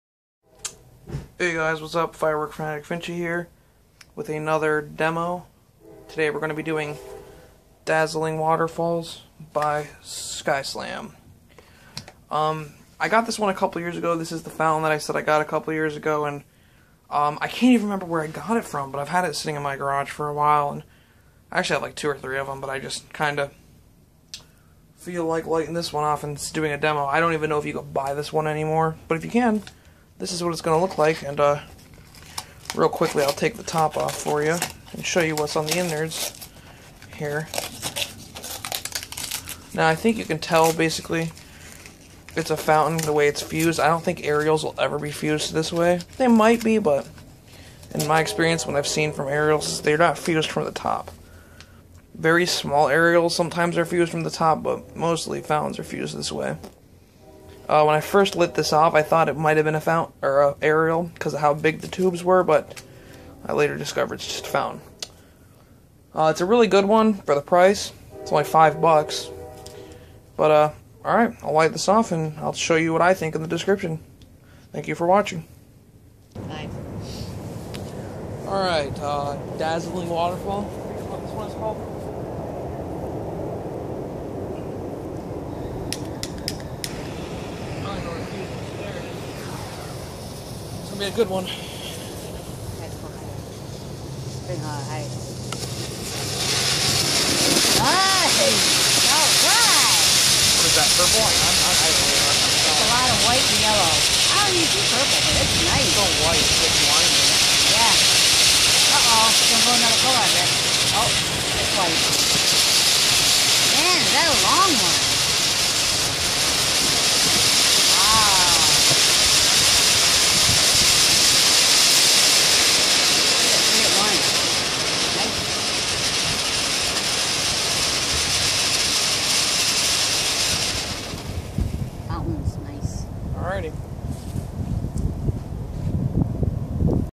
dazzling waterfall fountain - sky slam
6 out of 10
Reason for rating: this fountain was only 5 dollars and for the price it wasn't that bad but it definitily lacks any effects other then the generic spew forth spark effect